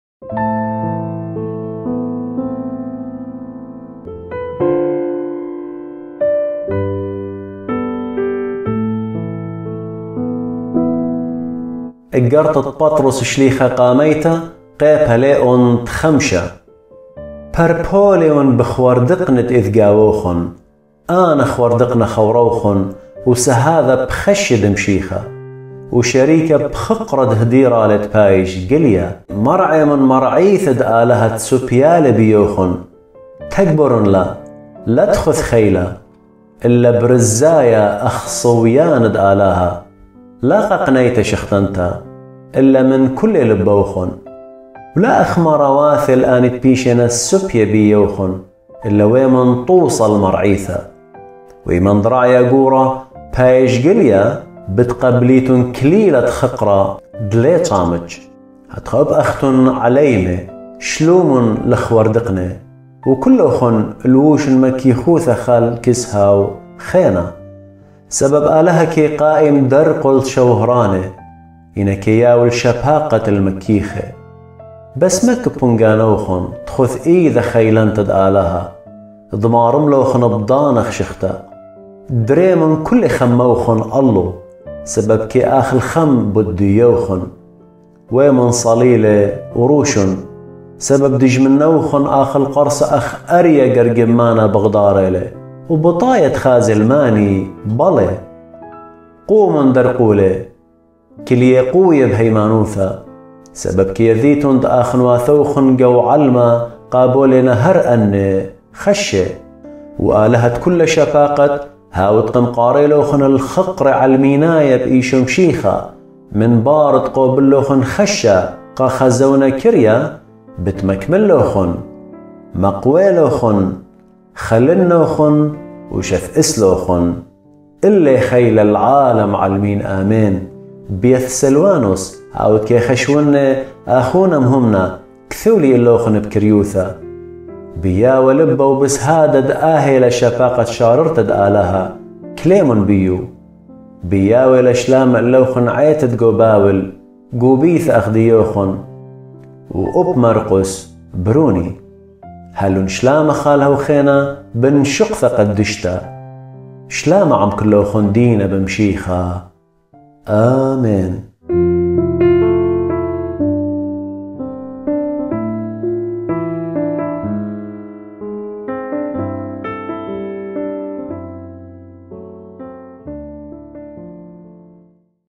1 Peter . Chapter 5. رسالة بطرس الأولى.الأصحاح ٥.
‏‎قراءة صوتية للكتاب المقدس ( العهد الجديد ) باللغة الآشورية.

‏Audio reading from the Bible( New Testament  )in Assyrian Language.

‏رسالة بطرس الأولى.
الأصحاح ٥.

ܐܓܪܬܐ ܕܦܛܪܘܿܣ ܩܕ݇ܡܝܬܐ.
ܩܦܠܐܘܢ  ܗ.

1 Peter . Chapter 5.

بصوت الشماس: روميل سوره.
ܒܩܠܐ ܕܡܫܡܫܢܐ : ܪܘܡܝܠ ܣܘܪܐ.